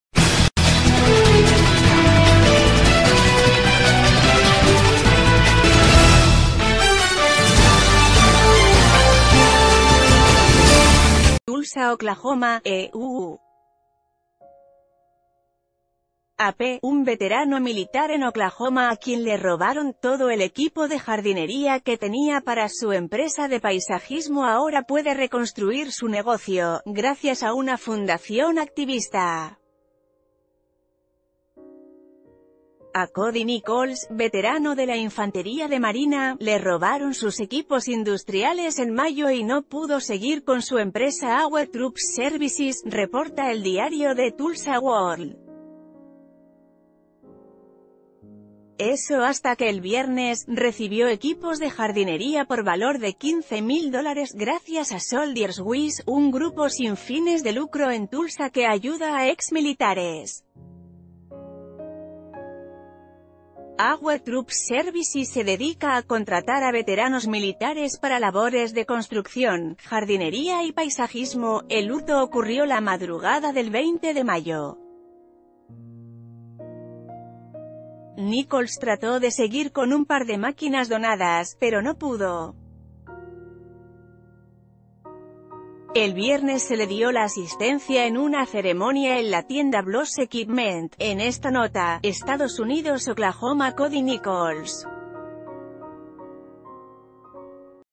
Rescatan negocio de veterano de Oklahoma tras hurto
TULSA, Oklahoma, EE.UU. (AP) — Un veterano militar en Oklahoma a quien le robaron todo el equipo de jardinería que tenía para su empresa de paisajismo ahora puede reconstruir su negocio, gracias a una fundación activista. A Cody Nichols, veterano de la infantería de marina, le robaron sus equipos in...